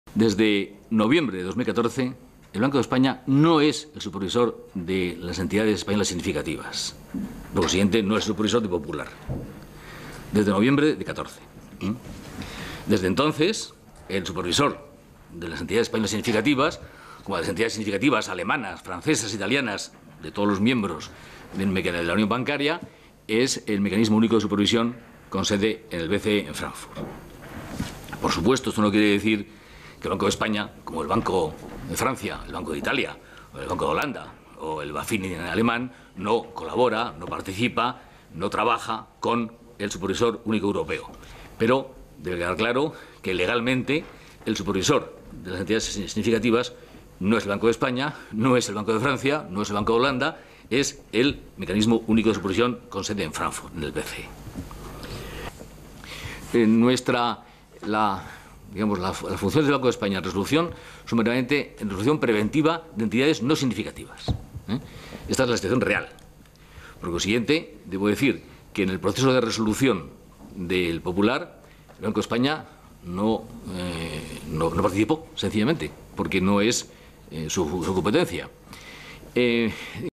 Linde elude responsabilidades en la quiebra del Popular
El gobernador del Banco de España, Luis María Linde, ha eludido responsabilidades en la quiebra del Banco Popular, ya que ha defendido que la institución que dirige ni era supervisor de esta entidad ni tampoco tuvo poder en la decisión de venderlo al Banco Santander por un euro.